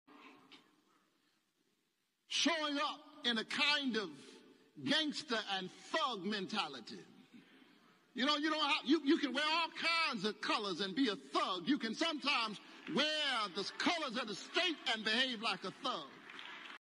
Raphael Warnock Refers To Police Officers As Thugs
Georgia US Senator Raphael Warnock Refers to Police Officers as "THUGS"
Is this the man you want ?
Watch here only on CCE2.